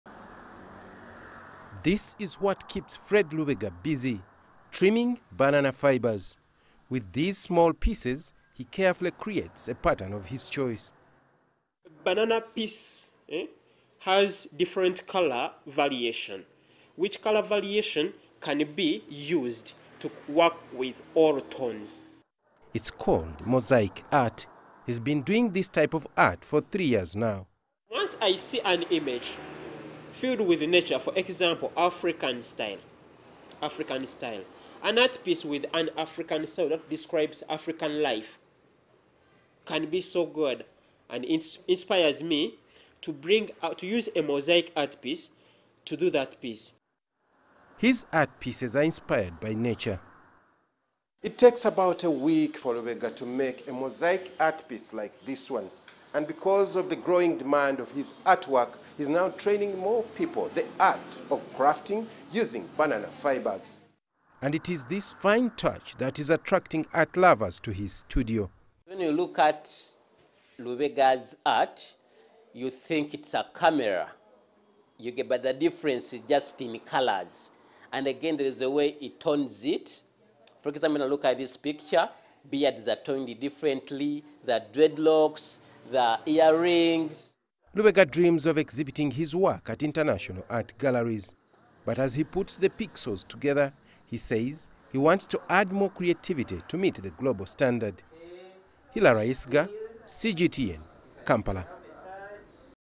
Ugandan artist uses banana fibres to create unique artwork
Let's change pace now and go to Uganda's art scene. One creative is using an unusual medium to create unique pieces that are drawing art lovers from far and wide.  Fred Lubega uses banana fibres to create mosaic portraits. It's a time-consuming process that requires infinite patience. CGTN's Hillary Ayesiga spent some time with Fred in his Kampala studio and filed this report.